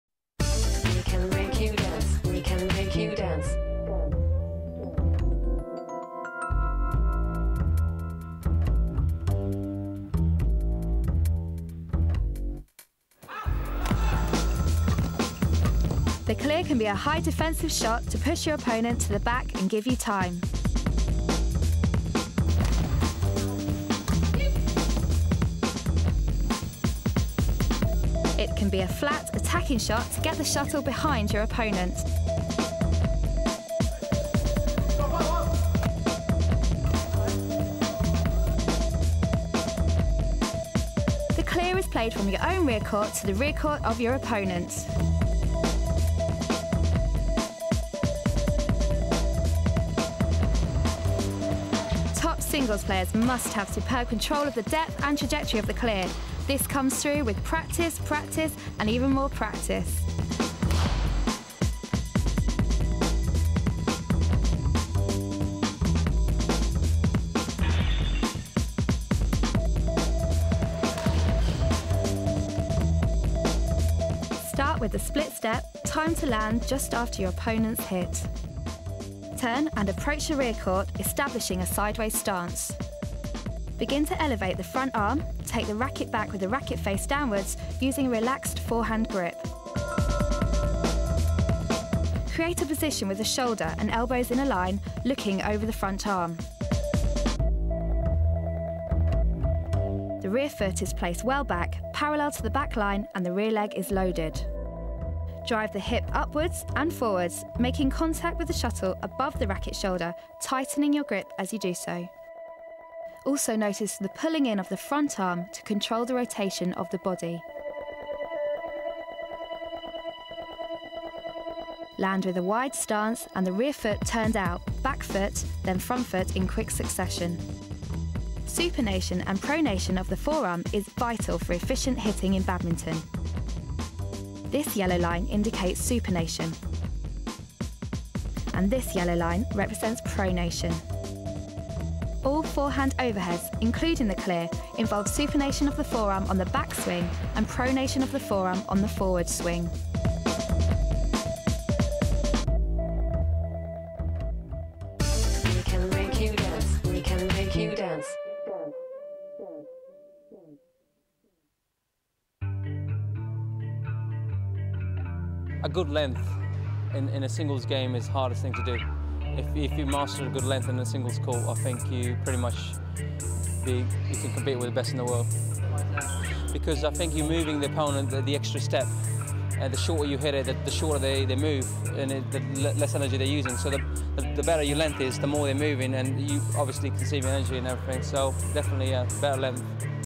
BADMINTON England - Technique DVD
Up your game this year with the BADMINTON England Technique DVD. This DVD will be invaluable to all levels of players who want to improve their game and coaches who are looking to develop their players in the best possible way.
The DVD has been shot by Century Television using high quality cameras that capture fantastic detail in slow motion. It features a detailed look at 19 of the most common strokes in badminton and includes footage of Nathan and Gail's build up to the Athens Olympics. It also features trick shots with Nathan and Rexy Mainaky and key tips from the players and national coaches.